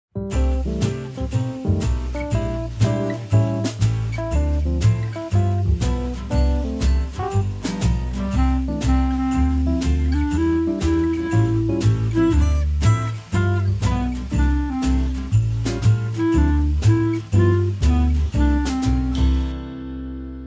Just whipped up some dreamy whipped cream with our Bosch mixer
It's like magic! 🍰✨ Pro tip: Chill the stainless steel bowl first for the fluffiest results. Trust me, it's a game changer!